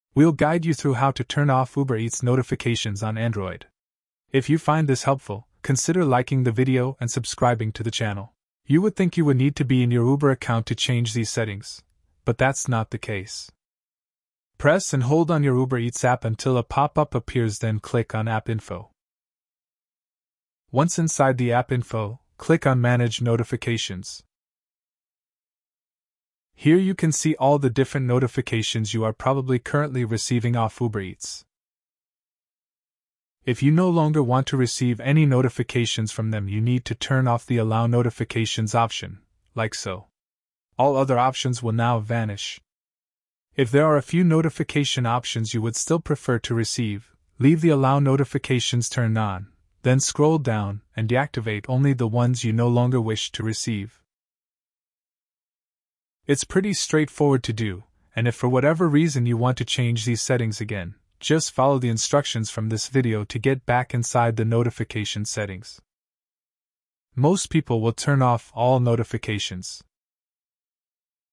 How To Turn Off Uber Eats Notifications On Android (Full Guide)
This video will show you how to turn off Uber Eats notifications on Android.